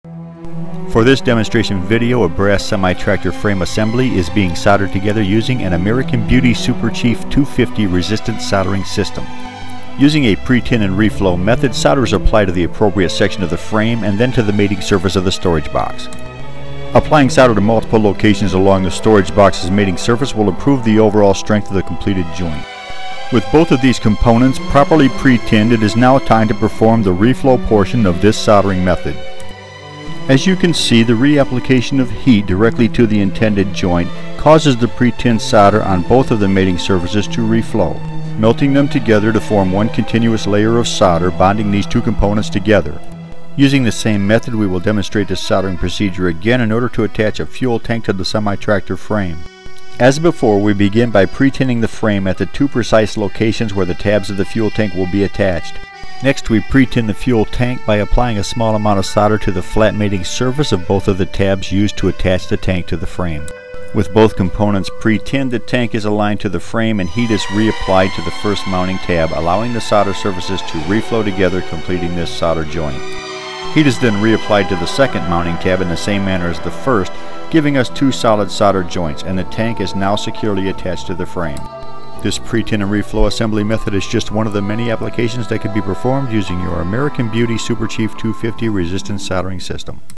Resistance Soldering Brass Modeling Components.
This video demonstrates resistance soldering brass components for a semi-tractor frame assembly using the American Beauty SC250 resistance soldering system.